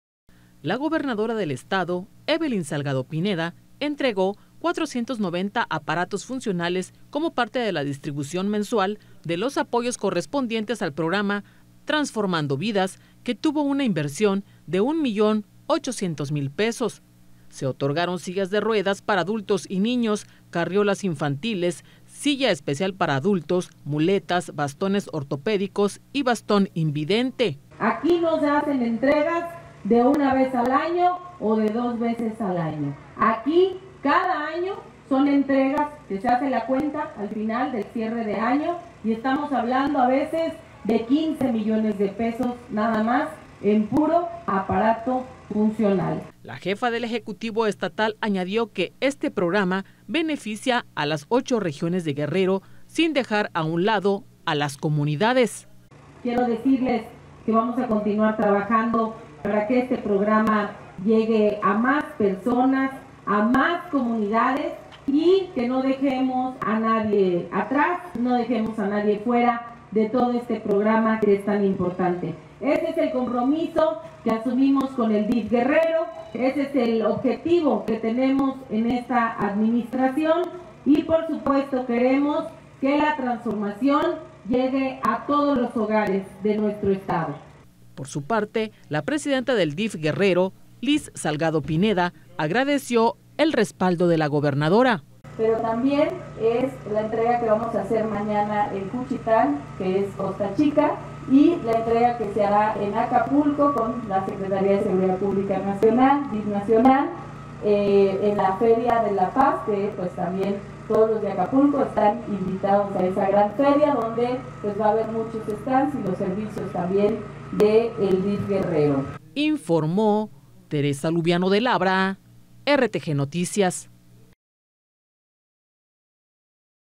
RTGnoticias | Entrega gobernadora aparatos funcionales del programa “Transformando Vidas”
La gobernadora del estado Evelyn Salgado Pineda entregó 490 aparatos funcionales como parte de la distribución mensual de los apoyos correspondientes al programa “Transformando vidas”, que tuvo una inversión de 1 millón 800 mil pesos.